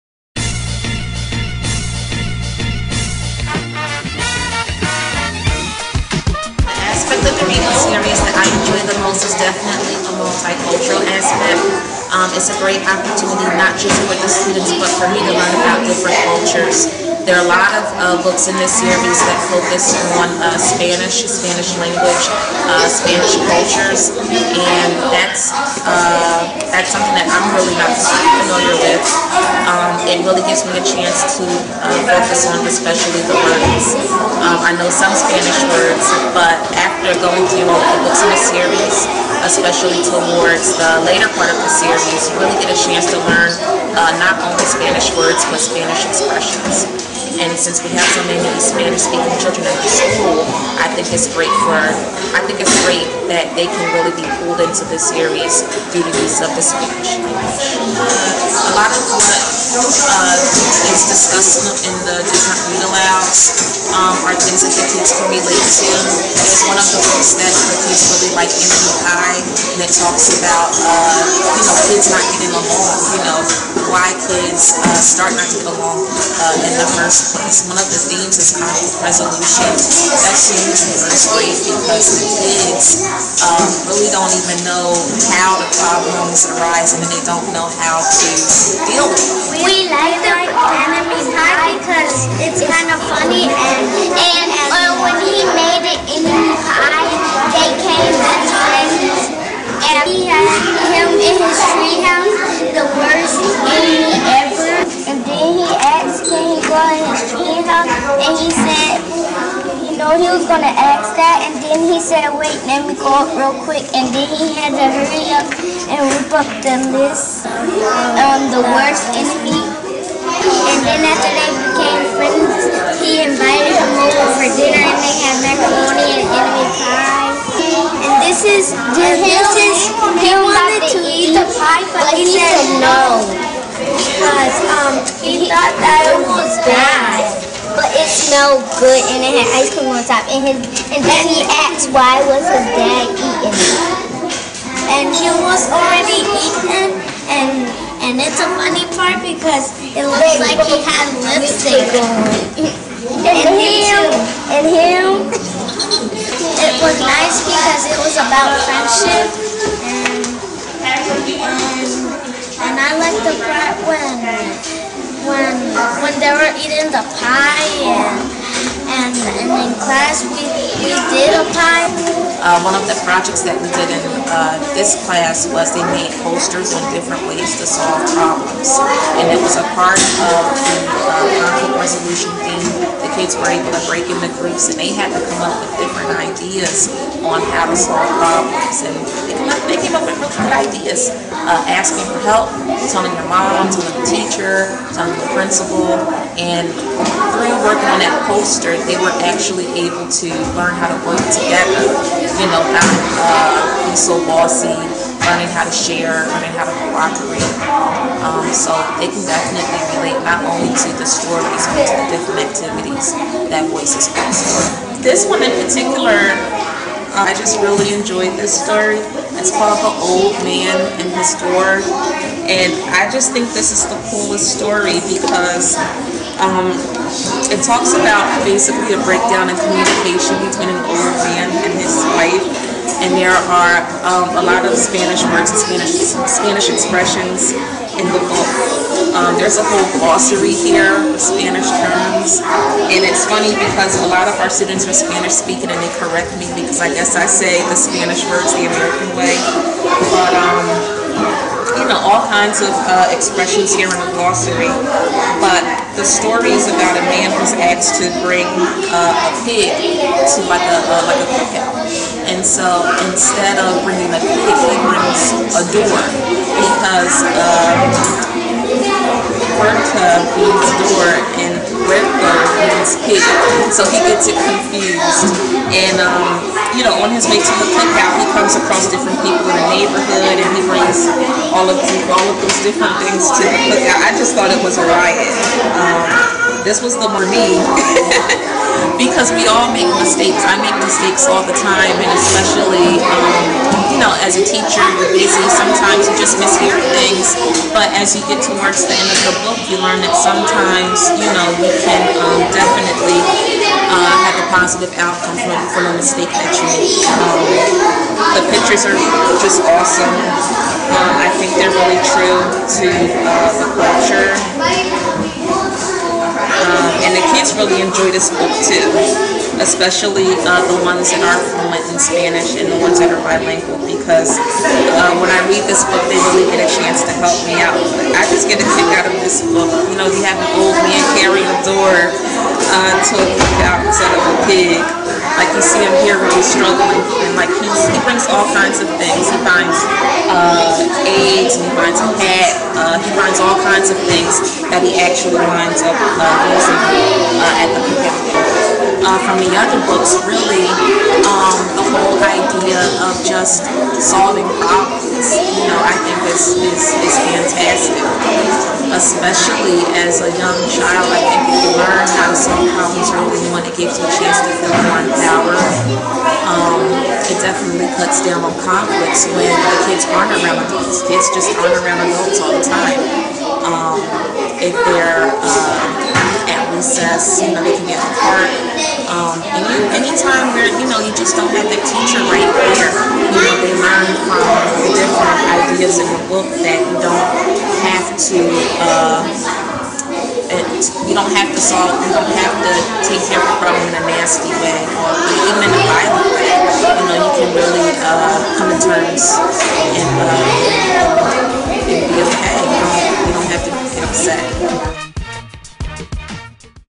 Voices Reading in Trenton, NJ - 1st grade
Voices Reading making impact at Washington School of Trenton, NJ